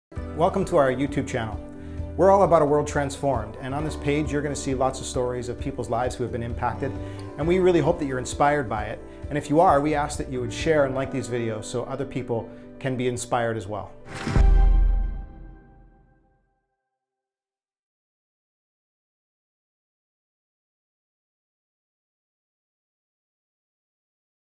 Welcome to HFL's YouTube channel
Find out who we are and what we do. Watch lives being impacted through our team videos. Hear life-changing stories from around the world.